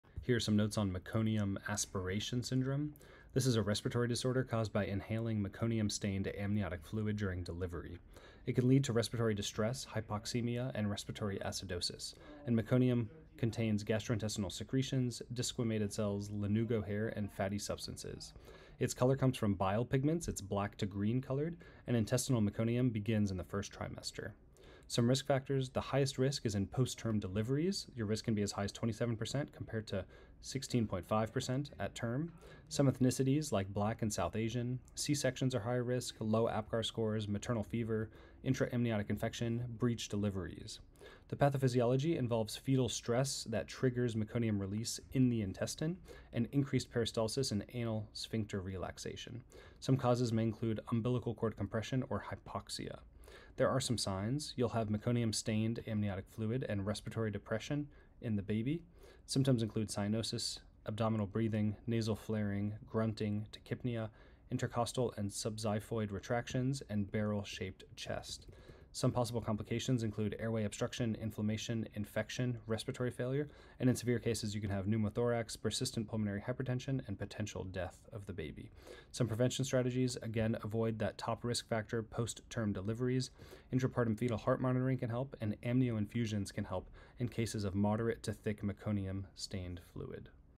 Meconium aspiration syndrome
- A respiratory disorder caused by inhaling meconium-stained amniotic fluid during delivery
- Can lead to respiratory distress, hypoxemia, and respiratory acidosis
- Meconium contains gastrointestinal secretions, desquamated cells, lanugo hair, and fatty substances
- Meconium color comes from bile pigments (black-green)
- Intestinal meconium begins in the first trimester

Risk factors
- Postterm deliveries (highest risk, 27% vs. 16.5% at term)
- Black race and South Asian ethnicity
- Cesarean section
- Low Apgar scores
- Maternal fever
- Intraamniotic infection
- Vaginal breech delivery

Pathophysiology:
- Fetal stress triggers meconium release in the intestine
- Increased peristalsis and anal sphincter relaxation
- Causes may include:
  - Umbilical cord compression
  - Hypoxia

Signs: Meconium-stained amniotic fluid; Respiratory depression
- Symptoms include: Cyanosis; Abdominal breathing; Nasal flaring; Grunting; Tachypnea; Intercostal and subxiphoid retractions; Barrel-shaped chest

Potential Complications:
- Airway obstruction
- Inflammation
- Infection
- Respiratory failure
- Severe cases can lead to: Pneumothorax;  Persistent pulmonary hypertension; Potential death

Prevention Strategies:
- Avoid postterm deliveries
- Intrapartum fetal heart monitoring
- Amnioinfusion (in cases of moderate to thick meconium-stained fluid)